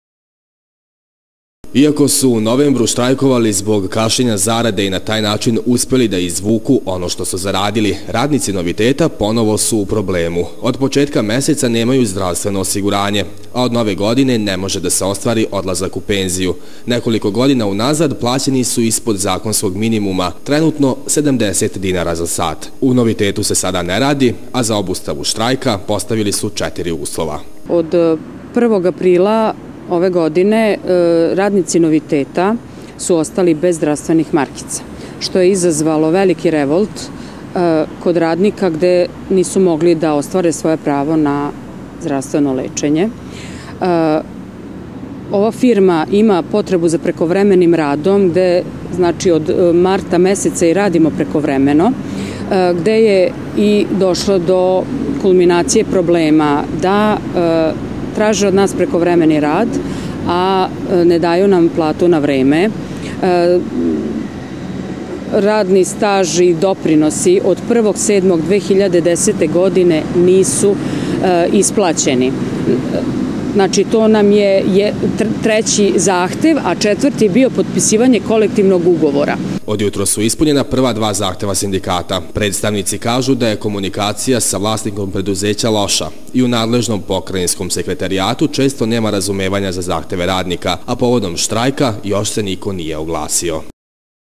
TV KANAL 9, NOVI SAD, Štrajk u Novitetu
Radnici u novosadskom "Novitetu" u potpunosti obustavili rad... razlozi poznati nema plata, a ni zdravstvenog osiguranja.